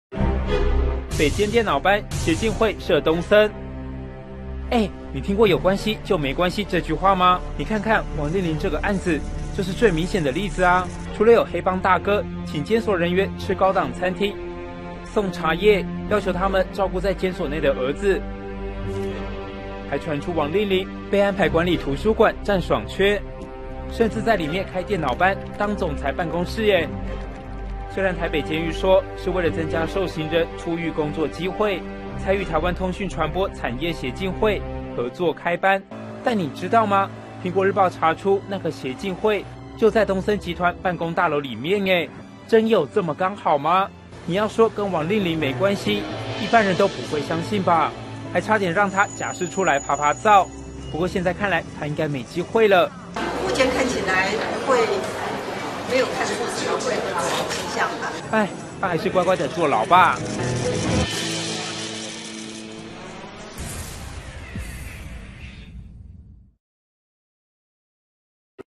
北監電腦班 「協進會」設東森--蘋果日報 20141120
【綜合報導】檢調查辦獄政史上最大貪瀆弊案，傳出東森前總裁王令麟在台北監獄服刑還能把電腦班（指媒體班）教室當成總裁辦公室，北監澄清，純為增加受刑人出獄工作機會，才與台灣通訊傳播產業協進會合辦「新媒體技能訓練班」，但《蘋果》昨查訪發現，協進會就位在東森集團辦公大樓，此外，北監還曾透過王募得價值七十一萬元的攝影設備，檢調將追查王是否利用人脈資源，換取特權待遇。

北監電腦班
「協進會」設東森
欸
你聽過「有關係」就沒關係這句話嗎
你看看
王令麟的這個案子
就是最明顯的例子啊
除了有黑幫大哥
請監所人員吃高檔餐廳
送茶葉
要求他們照顧在監所內的兒子
還傳出王令麟
被安排管理圖書館占爽缺
甚至在裡面開電腦班
當總裁辦公室耶
雖然台北監獄說
是為了增加受刑人出獄工作機會
才與台灣通訊傳播產業協進會
合作開班
但你知道嗎
《蘋果日報》查出那個協進會
就在東森集團辦公大樓裡面耶
真有這麼剛好嗎
你要說跟王令麟沒關係喔
一般人都不相信吧
還差點讓他假釋出來趴趴走
不過現在看來
他應該沒機會了

目前看起來不會
沒有看出什麼會核准(假釋)的跡象吧

唉
他還是乖乖的坐牢吧